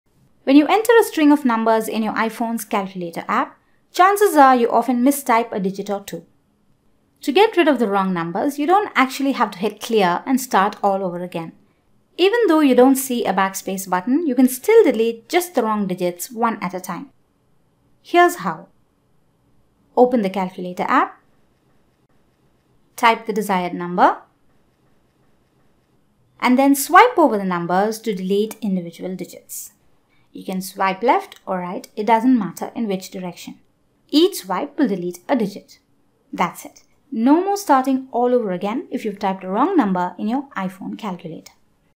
How to Delete Digits in the Calculator App
Learn how to remove individual digits in the Calculator app on your iPhone.